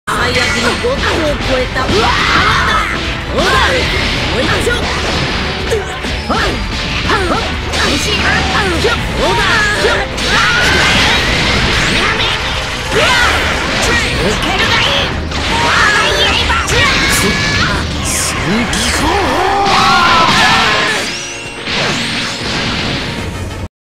Non sparking TOD - Bluku
Just playing around with improved Goku SSGSS and found this fun combo.